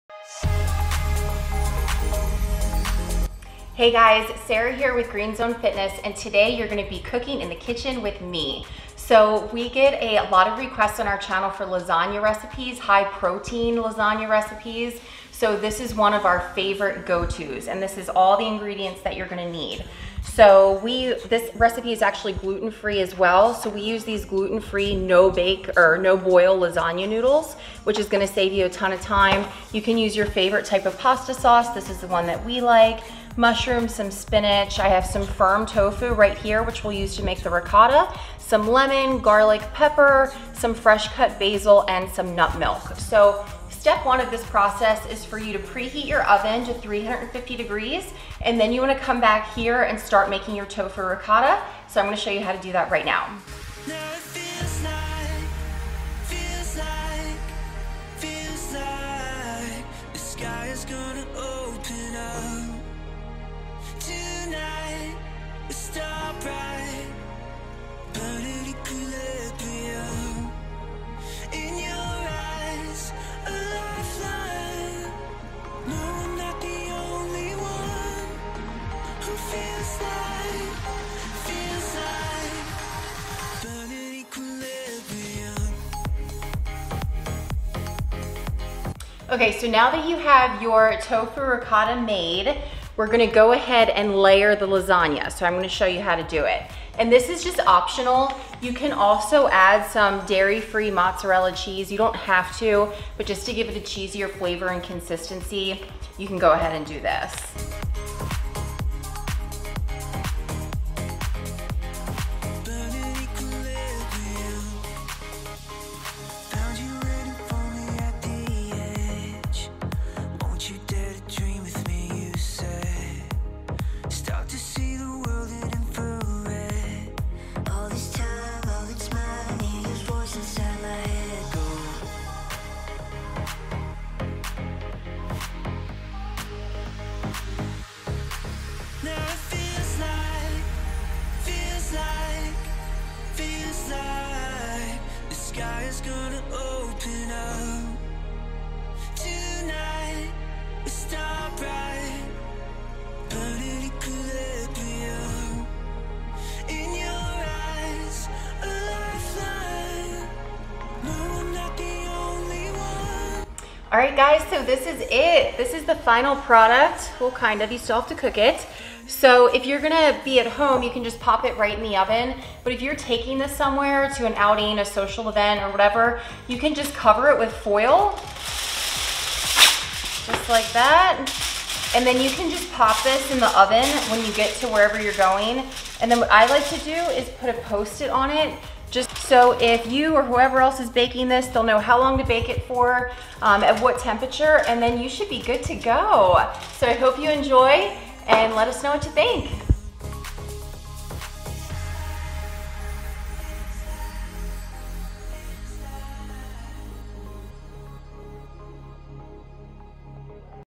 Tofu Spinach Mushroom Lasagna
This lasagna is so delicious, no one will even know it's dairy-free AND gluten-free! It is much lower in calories, fat and cholesterol than traditional lasagna dishes. The tofu ricotta is a much healthier alternative to ricotta cheese and only requires 6  ingredients. It is great for this lasagna dish, a variety of other Italian dishes or you can use it as a spread for toast or crackers. This is an easy meal to make for busy weeknight dinners or meal prep... and the leftovers are delicious!

Sara IG - @buffplantbasedblonde
Ryan IG - @theplantbasedbeast